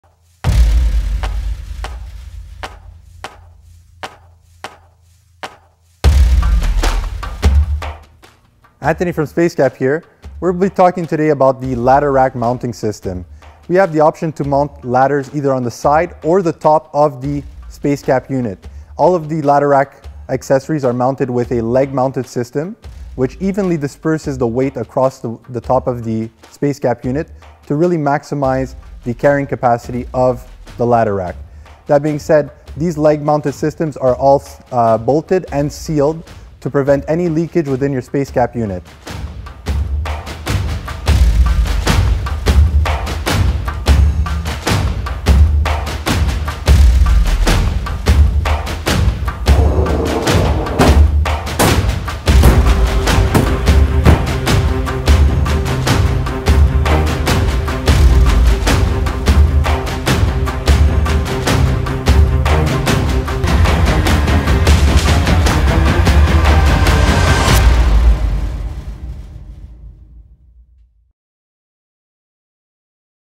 SpaceKap Features: Episode 19 - Ladder Rack
SpaceKap offer side or top mounted ladder racks in various models and configuration. Each mounted leg is bolted and sealed to prevent any leakage inside the unit and give you easy access to your tools.